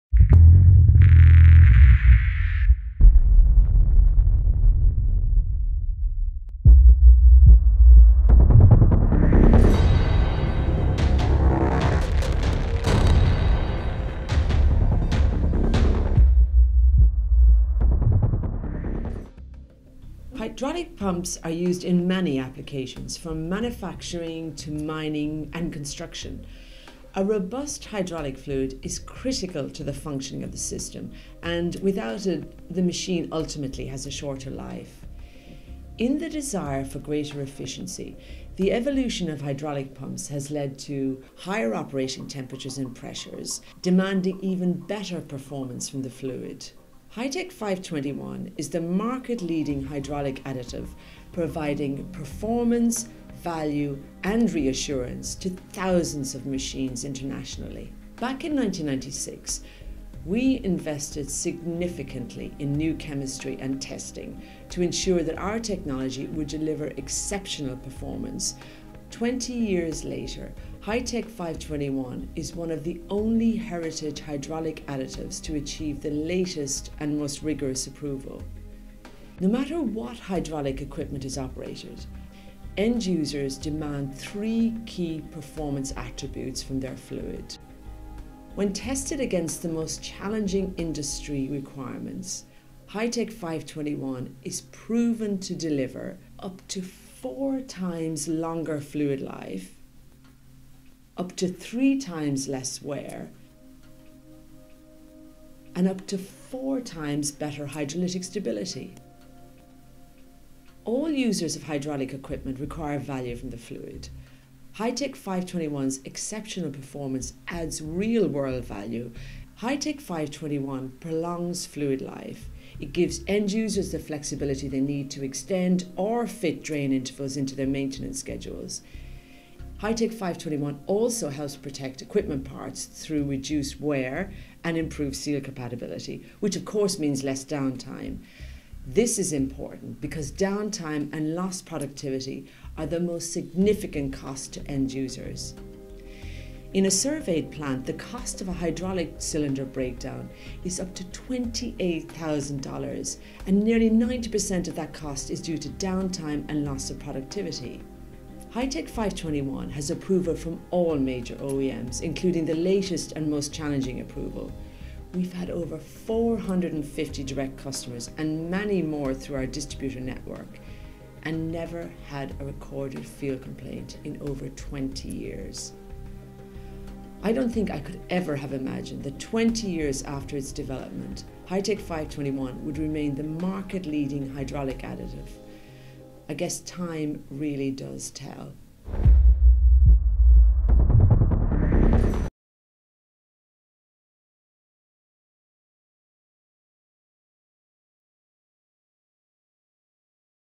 Afton Chemical - HiTEC 521
HiTEC® 521 is one of a kind: not only does it meet the latest, most challenging OEM requirements, it also comes with the reassurance of over 20 years in the field without complaint.

Hydraulic pumps are used in many applications, from manufacturing, to mining and construction. A robust hydraulic fluid is critical to the functioning of the system and without it, the machine simply fails to operate.

In the fight for efficiency, hydraulic pumps have evolved. Hydraulic fluids and the additives inside must now perform under even higher operating temperatures and even greater pressure.